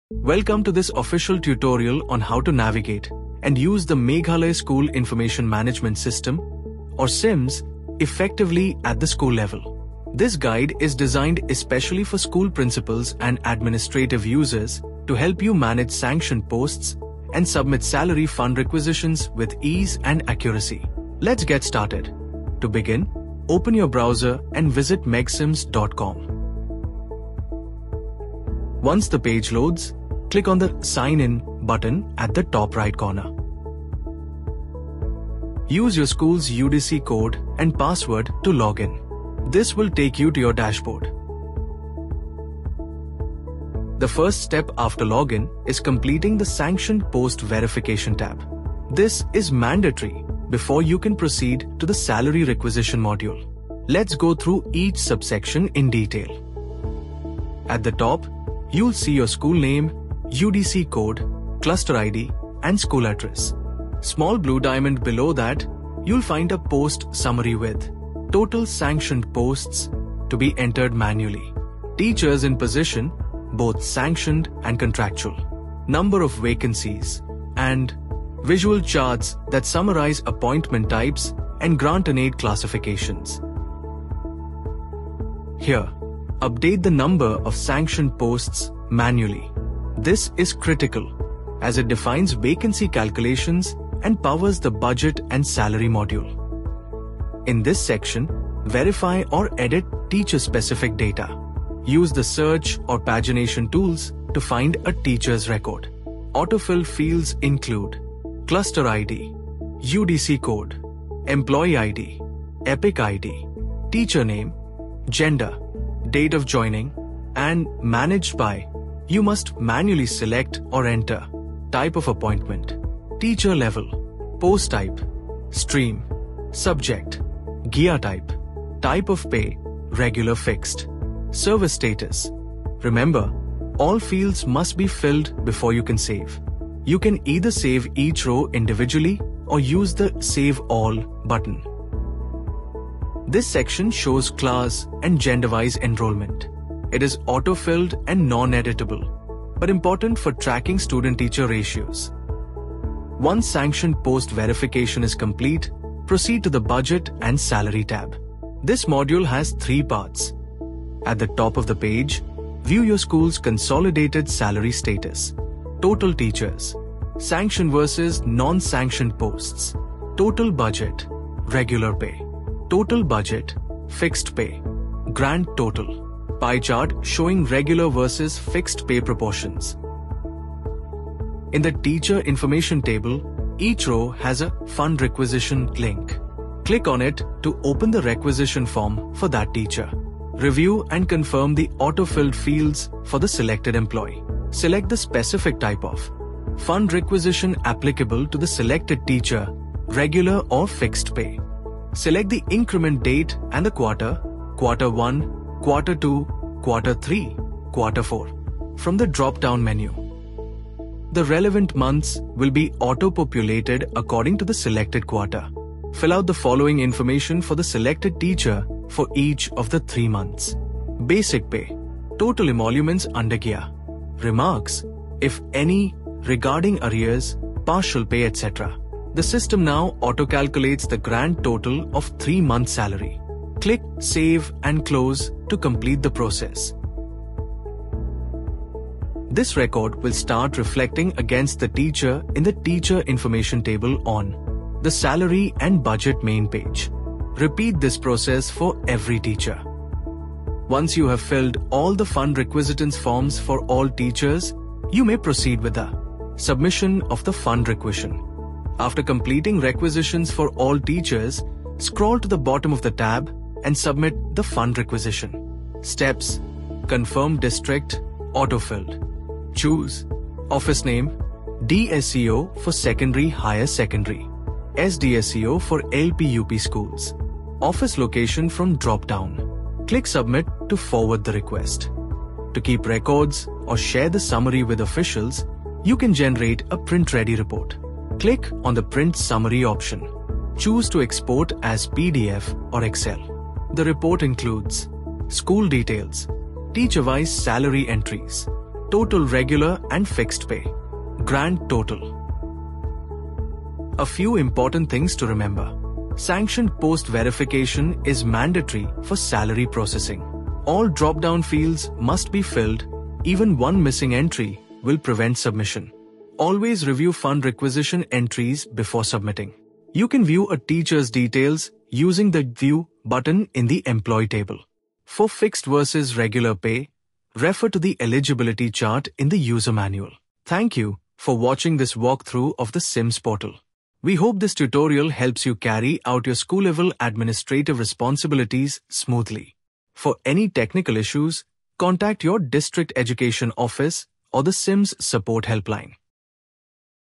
Steps on how to submit Budget and Salary in MLENS portal
This Video is intended for users logging into the Meghalaya School Information
Management System (MEG-SIMS) at the school level. It covers login procedures, sanctioned post
verification, and fund requisition for salary processing. The goal is to ensure data accuracy and
timely submission of salary requirements to the district administration.
Via the principal login, schools will initiate the Submission of Salary for 3 months of each
teacher, and will then proceed with the Fund Requisition submission